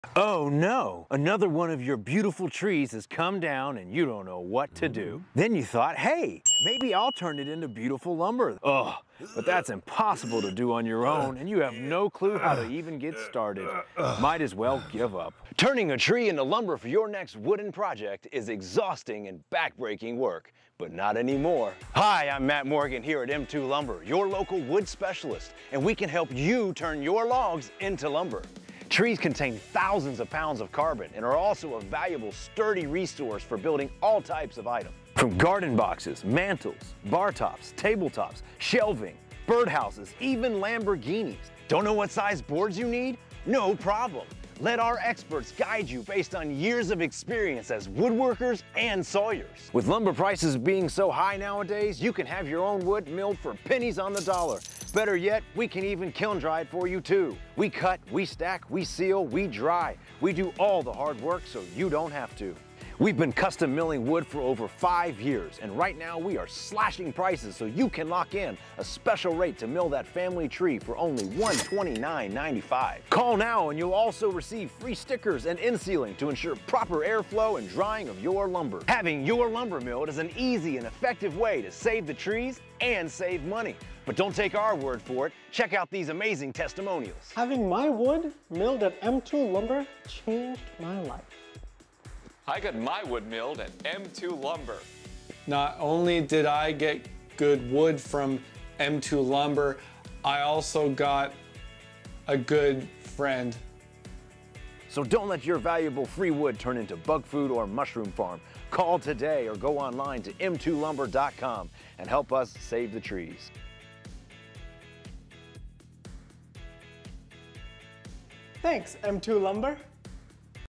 Turning your Logs into Lumber is Easy!
Hey Guys and Gals, in case you weren't already aware, we do offer custom milling for the public. Bring your logs down to us and have us turn them into beautiful slabs or boards for your next woodworking project?

We charge by the hour to mill logs due to such large variation in size that we receive, however, we can easily mill several hundred bdft/hr putting your milling costs under $1/bdft.

Also, after we've milled your wood you need to have it dried, why not have it dried here too!? Thats right, we mill, we dry, we even build furniture. We know exactly the best methods and techniques to mill wood from a woodworkers perspective to maximize output and quality from your logs.

*Special thanks to a few friends that helped make this video :)